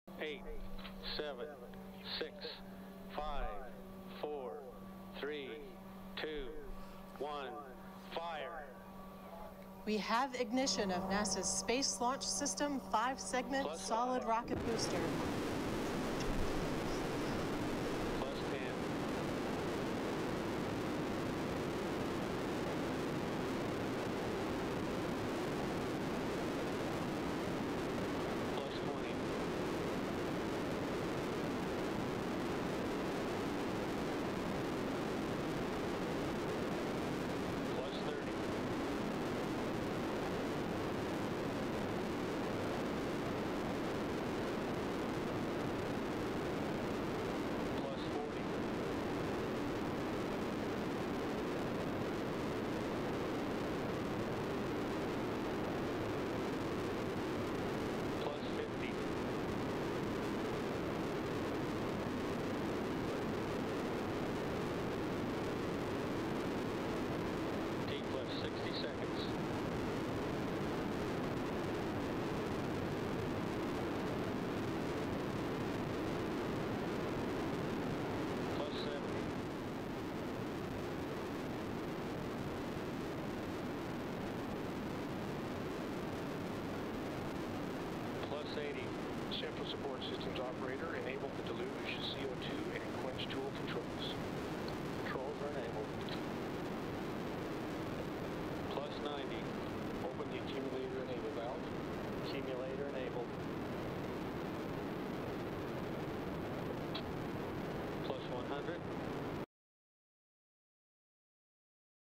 Orbital ATK Space Launch System SRB QM-1 Ground Test Firing
Orbital ATK Space Launch System SRB Qualification Motor QM-1 Ground Test Firing March 11, 2015